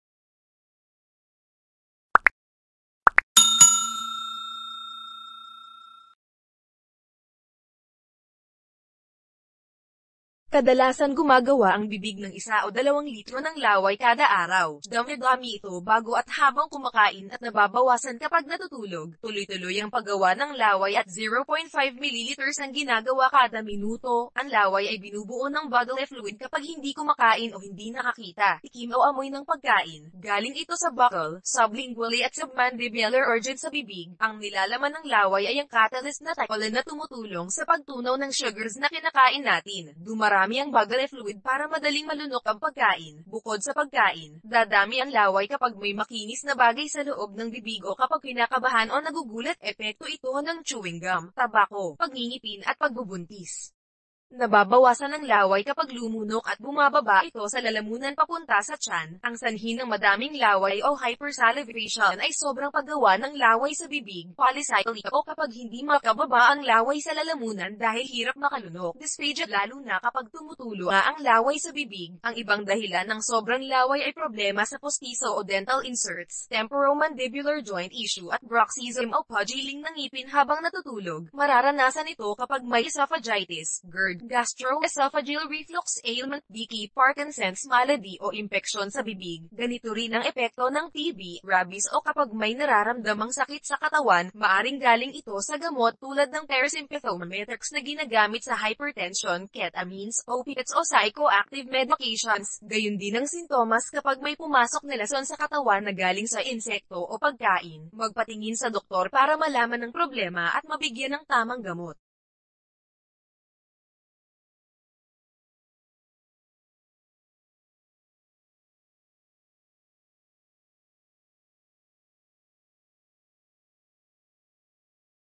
Ang sanhi ng mada­ming laway
Siomai King Online Shop door to door delivery order na click the
Kadalasan gumagawa ang bibig ng 1 o 2 litro ng laway kada-araw. Duma­dami ito bago at habang kumakain at nababawasan kapag natutulog. Tuluy-tuloy ang paggawa ng laway at 0.5 milliliters ang ginagawa kada-minuto.

Ang laway ay binubuo ng bodily fluid kapag hindi kumakain o hindi nakakita, tikim o amoy ng pagkain. Galing ito sa buccal, sublingual at submandibular organs sa bibig. Ang nilalaman ng laway ay ang compound na ptyalin na tumutulong sa pagtunaw ng starches na kinakain natin. Dumarami ang bodily fluid para madaling malunok ang pagkain.

Bukod sa pagkain, dadami ang laway kapag may makinis na bagay sa loob ng bibig o kapag kinakabahan o nagugulat. Epekto ito ng chewing­ gum, tabako, pagngi­ngipin at pagbubuntis.

Nababawasan ang laway kapag lumunok at buma­baba ito sa lalamunan papunta sa tiyan.

Ang sanhi ng mada­ming laway o hypersalivation ay sobrang paggawa ng laway sa bibig (polysialia) o kapag hindi makababa ang laway sa lalamunan dahil hirap makalunok (dysphagia) lalo na kapag tumutulo ang laway sa bibig.

Ang ibang dahilan ng sobrang laway ay problema sa pustiso o dental inserts, temporomandibular joint issue at bruxism o paggiling ng ngipin habang natutulog, Mararanasan ito kapag may esophagitis, GERD (gastroesophageal reflux illness), biki, Parkinson's infection o impeksiyon sa bibig.

Ganito rin ang epekto ng TB, rabies o kapag may nararamdamang sakit sa katawan. Maa­ring ga­ling ito sa gamot tulad ng parasympathomimetics na ginagamit sa hypertension, ketamines, opiates o psycho­active medications. Gayundin ang sintomas kapag may pumasok na lason sa katawan na ga­ling sa insekto o pagkain.

Magpatingin sa doktor para malaman ang problema at mabigyan ng tamang gamot.